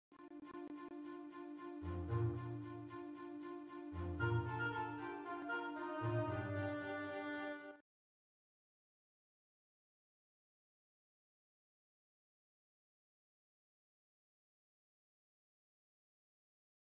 staring contest between cute blobs
cute little blobs having a staring contest!!